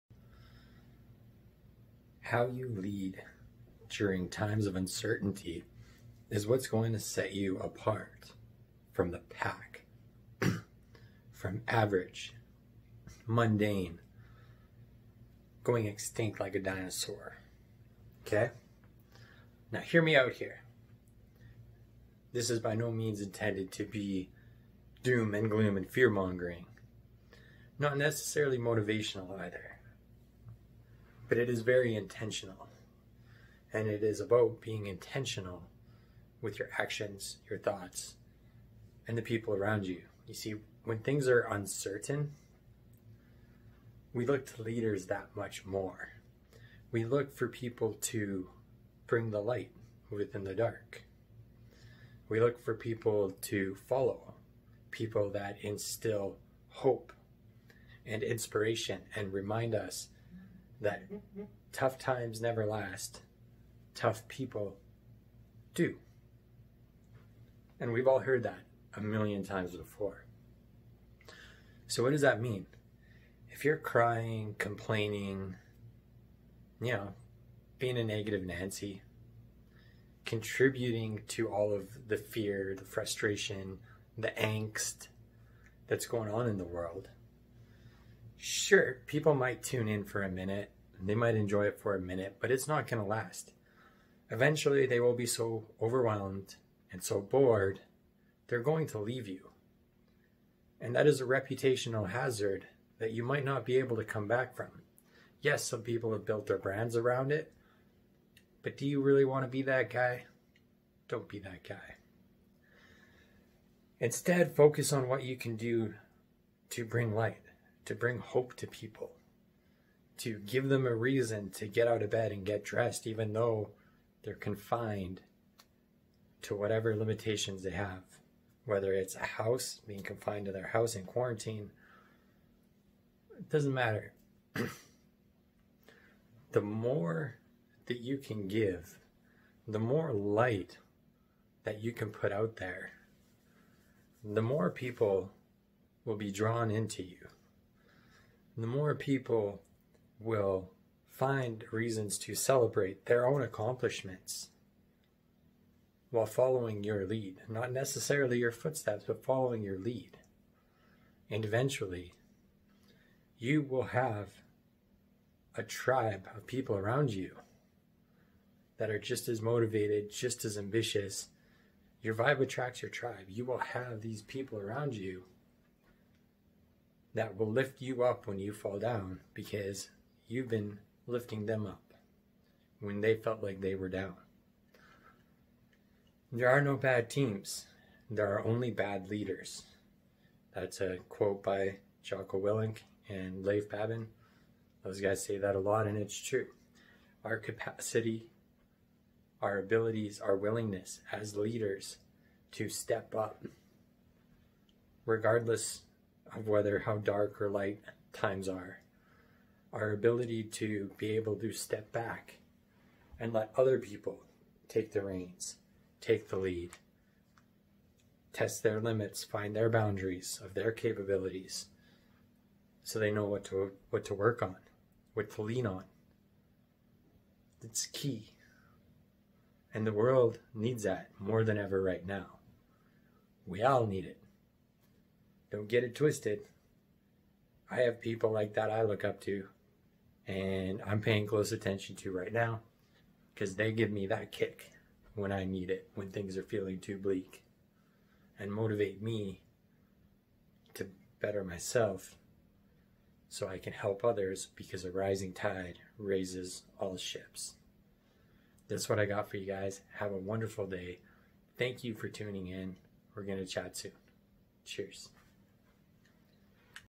HOW To Take The Lead And Win!
I feel like I'm ALWAYS off center on my videos. That's cool though.

How you lead, especially in times of contraction or struggle will set you apart from the pack in a hurry.

There are those that lead to capitalize and bring others down.

There are those who lead to lift others up, give their all to the world in everything they do, and provide abundant opportunities for others to find their wings, walk their path.

Which do you feel is more productive?

Which is more beneficial?

Why?

In general the world can be a dark place even when times are "good." When times seem dark they get amplified that much more.

Your ability to lead is determined in times of contraction.

People are drawn to light.

Be the light in all times